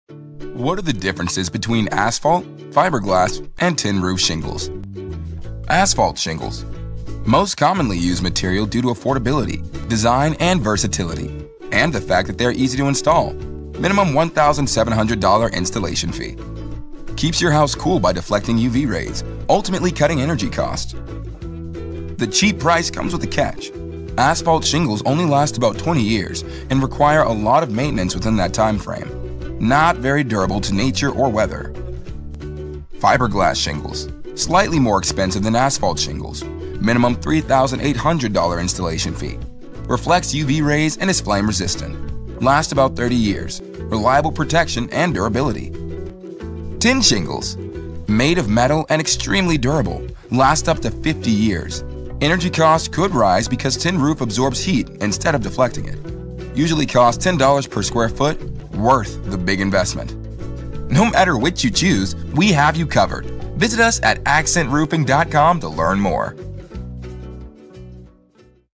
Accent Differences Between Asphalt, Fiberglass, and Tin Roof Shingles
Visit us for more tips and tricks on how you can protect your home with an improved roof. accentroofing.com/